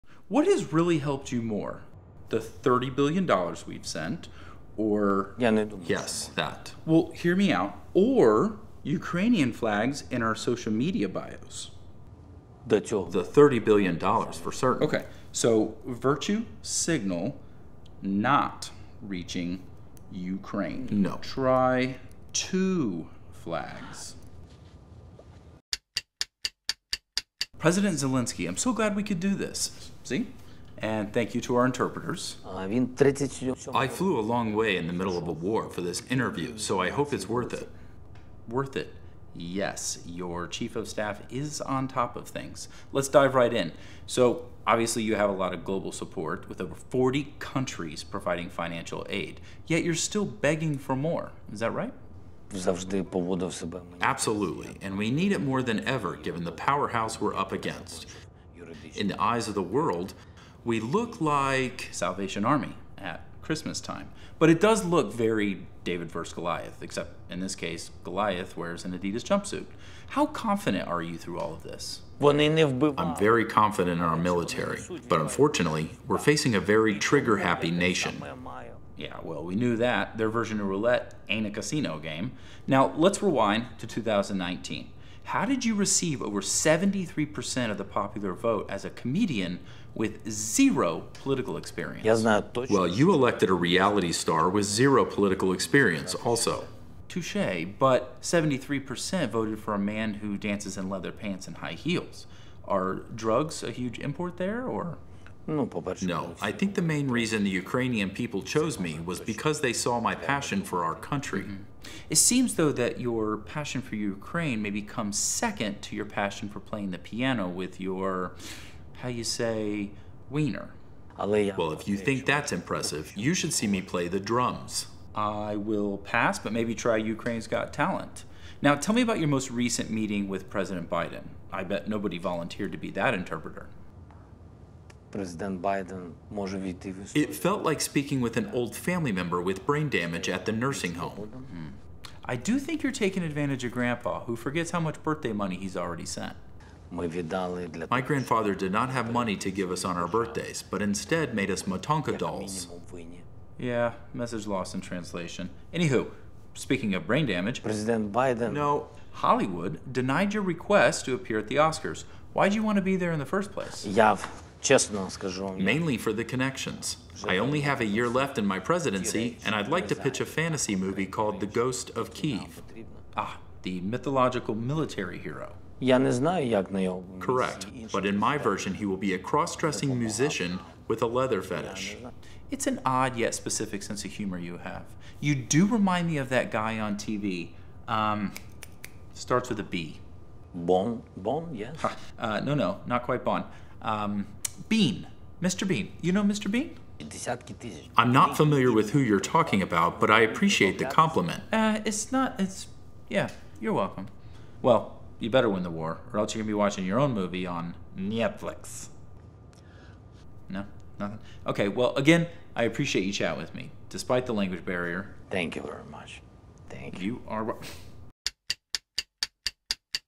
3 Minutes: Interview with Volodymyr Zelenskyy
3 Minutes:  Interview with Volodymyr Zelenskyy
I sat down with Volodymyr Zelenskyy to ask him about his election, if he needs more financial aid, and about playing the piano with his wiener.